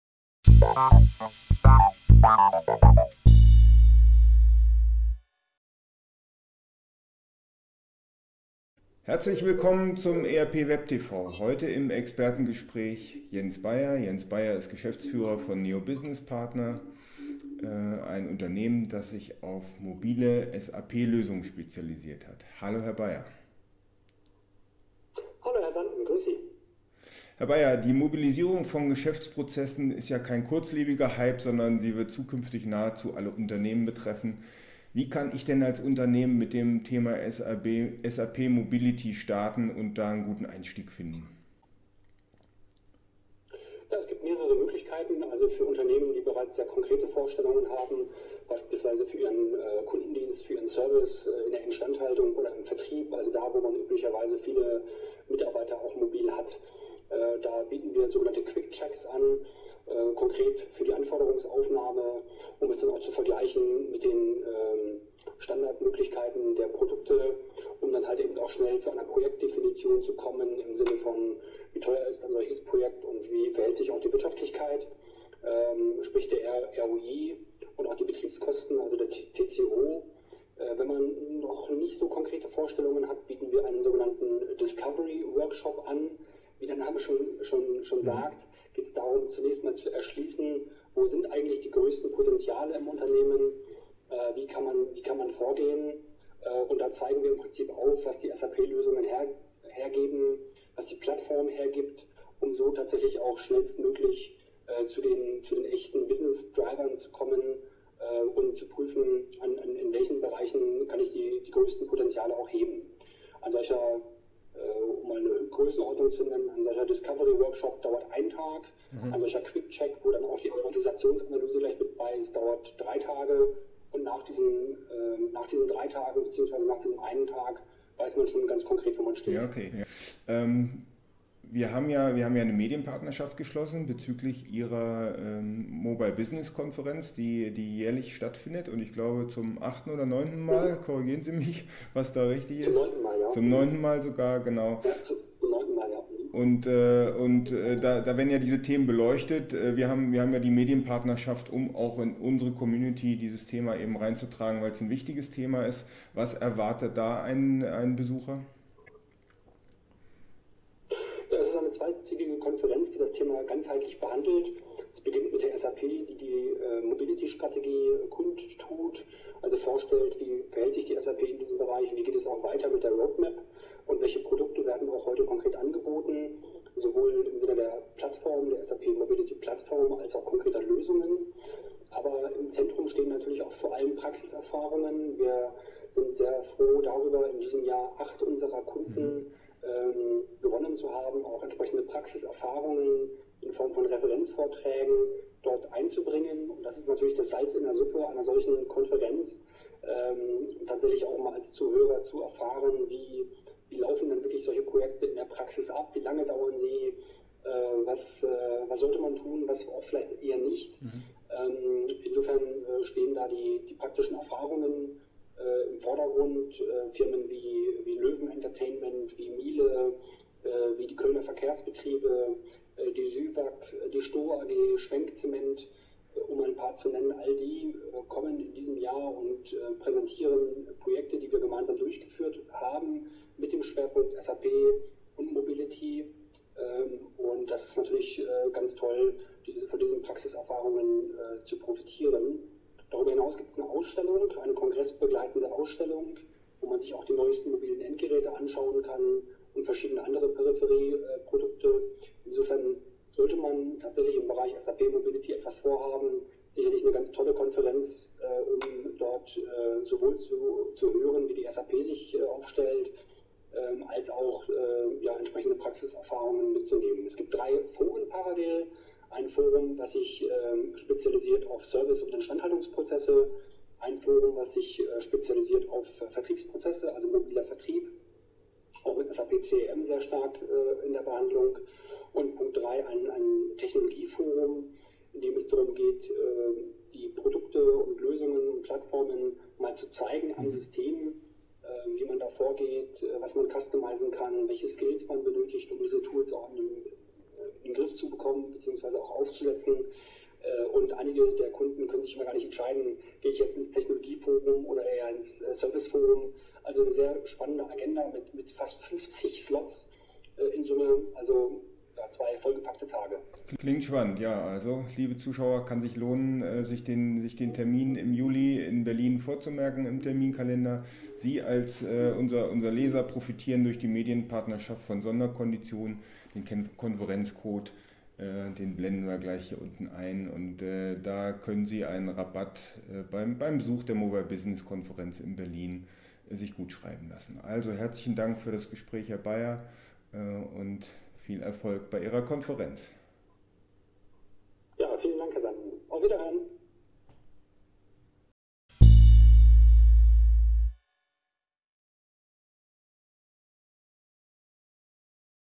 ERP webTV: Expertengespräch mit Jens Beier zur NEO Mobile Business Conference 2013
Interview mit Jens Beier, NEO Business Partners, zur Mobile Business Conference 2013 am 01. und 02. Juli 2013 in Berlin. Alle Zuseher profitieren von Sonderkonditionen beim Besuch der Konferenz.

Im ERP web TV befragten wir Jens Beier, wie SAP Anwendern ein guter Einstieg in "Mobile Lösungen" gelingt und was die Besucher der Mobile Business Conference erwartet.

Das Interview führte Helge Sanden, Chefredakteur des IT-Onlinemagazins, im ERP web TV.

ERP web TV - powered by IT-Onlinemagazin